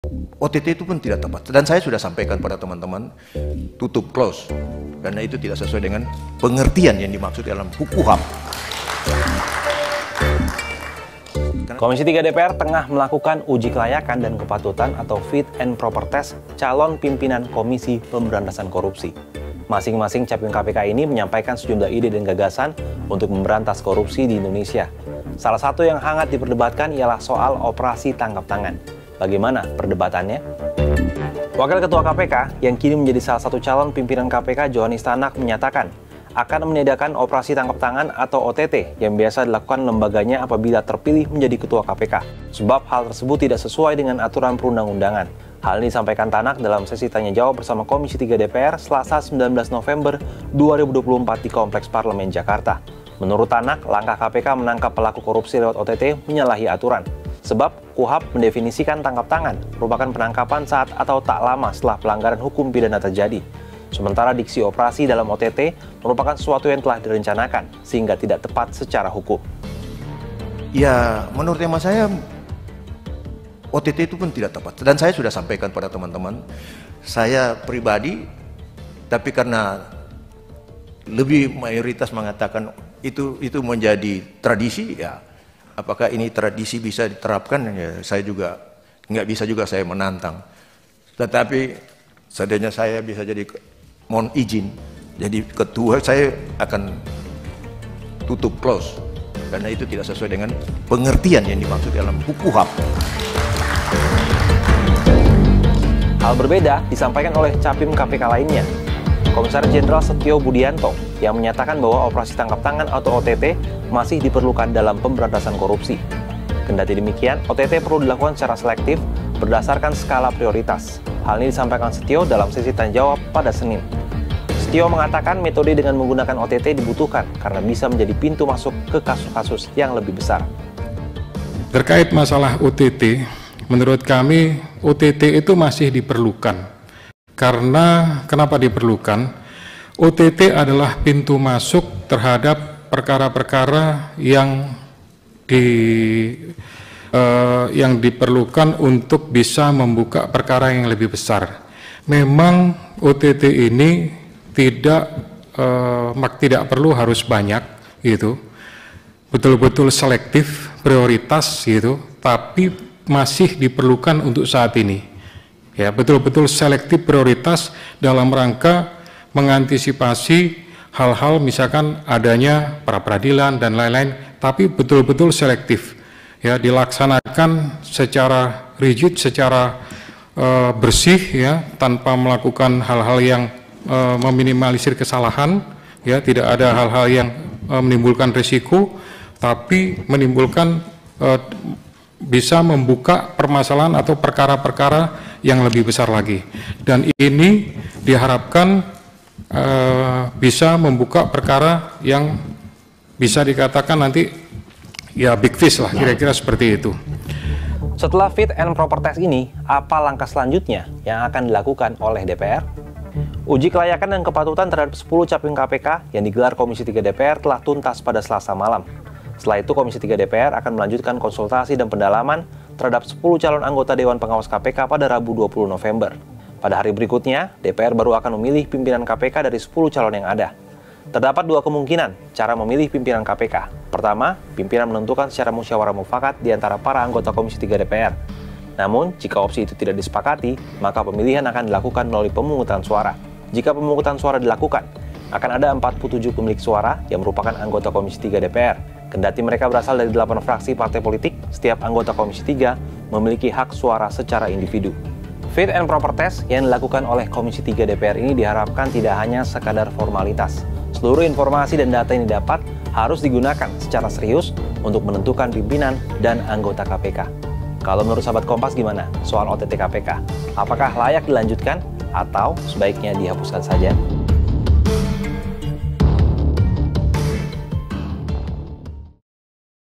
Operasi Tangkap Tangan Jadi Isu Panas Para Calon Pimpinan KPK
Komisi III DPR tengah melakukan uji kelayakan dan kepatutan atau fit and proper test calon pimpinan Komisi Pemberantasan Korupsi. Masing-masing capim KPK ini pun menyampaikan sejumlah ide dan gagasan untuk memberantas korupsi di Indonesia. salah satu yang hangat diperdebatkan soal operasi tangkap tangan. bagaimana perdebatannya?